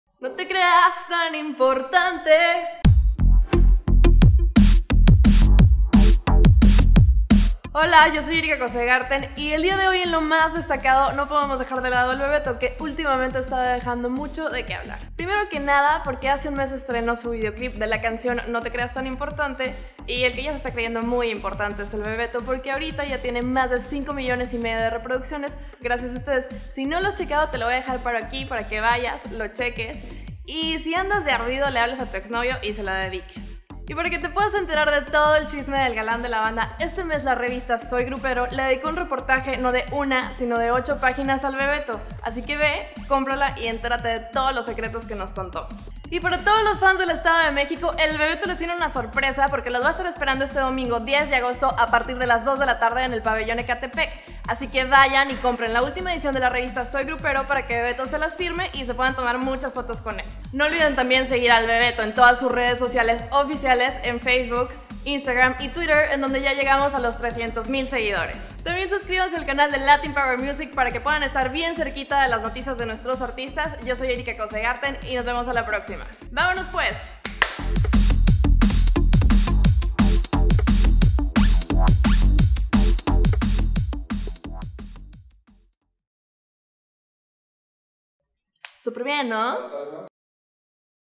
Latin Power Music [Lo + Destacado] - Revista Soy Grupero
Este mes la revista Soy Grupero le dedicó un reportaje de 8 páginas al Bebeto, no te pierdas la firma de la revista Soy Grupero con El Bebeto este domingo 10 de Agosto a las 2:00 pm en el Pabellón Ecatepec.

Revista SOY GRUPERO

Más Información sobre la firma de la revista en PABELLÓN ECATEPEC